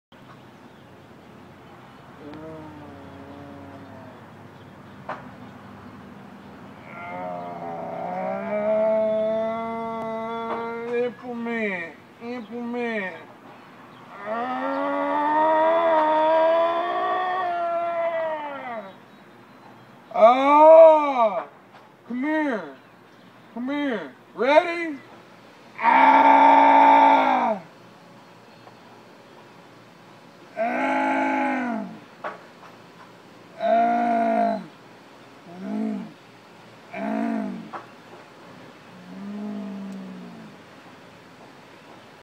Bum Getting High
Sniffing paint from can.